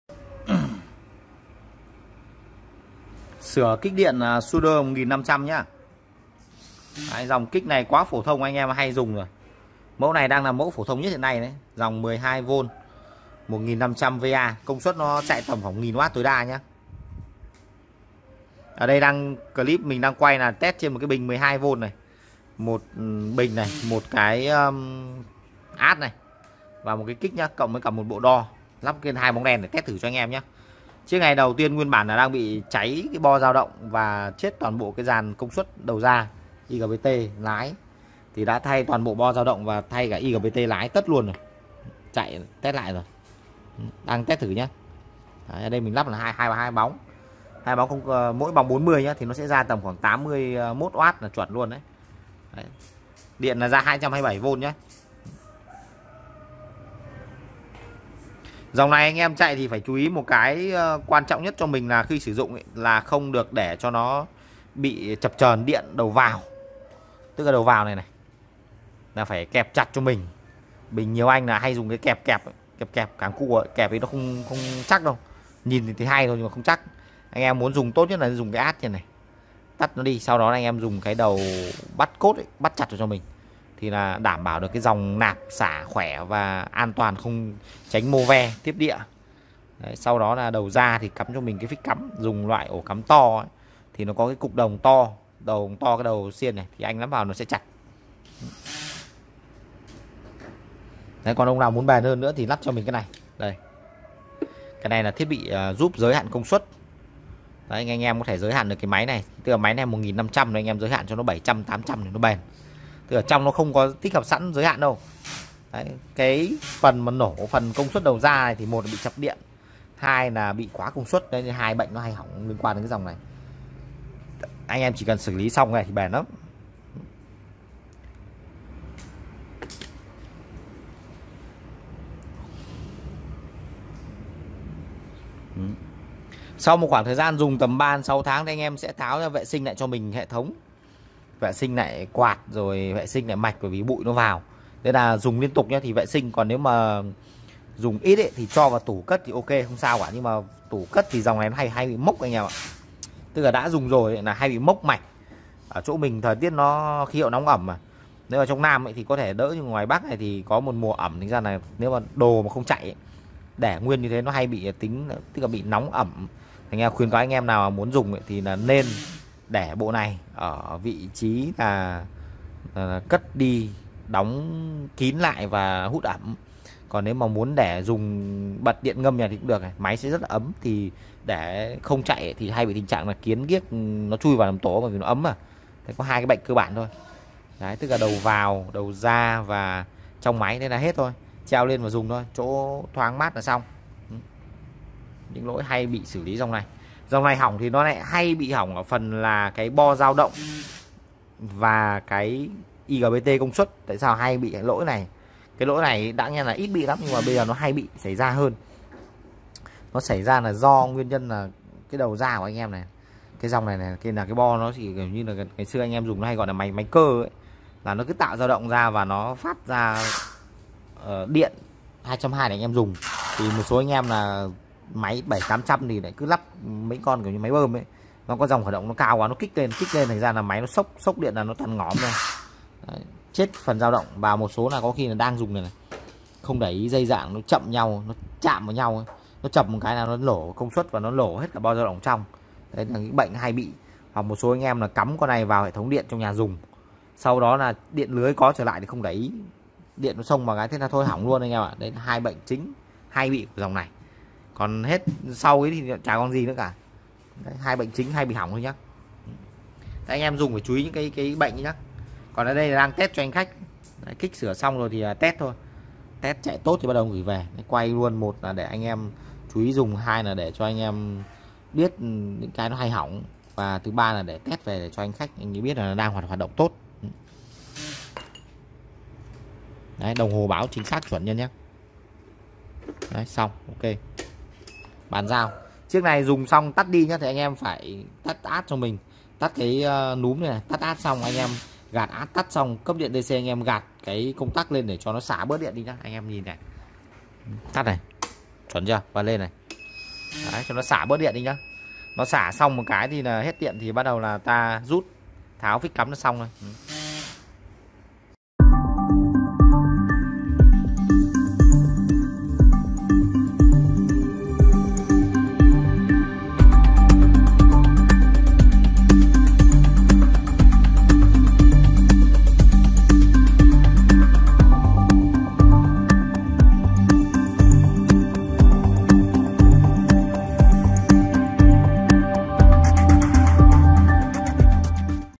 Sửa kích điện Suoer lỗi không ra điện - Repair Suoer Error Voltage
◅▷  Kích 1500ww sử dụng sai công suất hoặc đấu chạm chập dẫn tới lỗi bo dao động và bo công suất
Cách xử lý : thay cầu, Thay driver, thay bo sin

💓 Phụ Kiện Thiết Bị Điện Giá Buôn